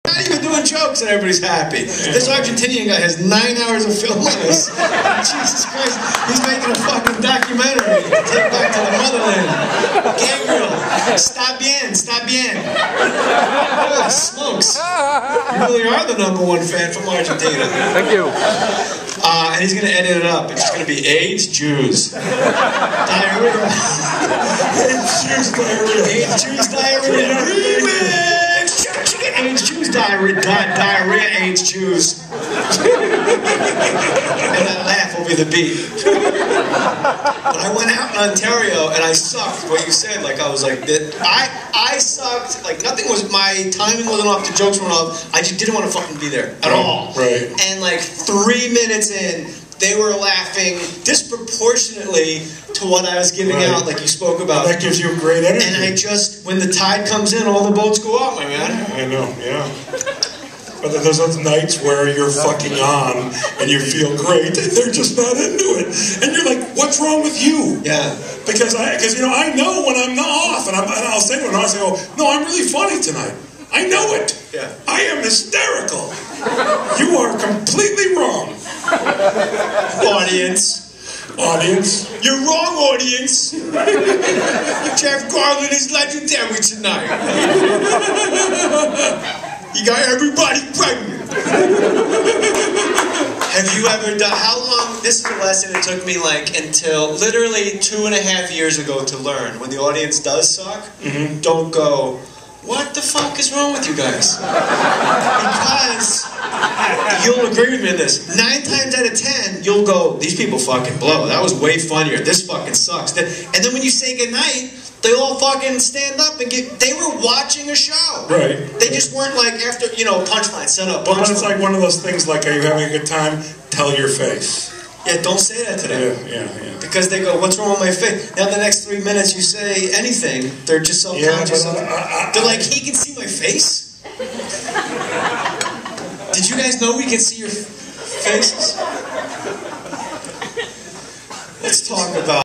Jay Mohr, Jeff Garlin en el Hollywood Improv 2
Acá Jay Mohr se acuerda del argentino que está en la sala grabando. Y opinan  sobre el público de stand up. Gran podcast, gran charla!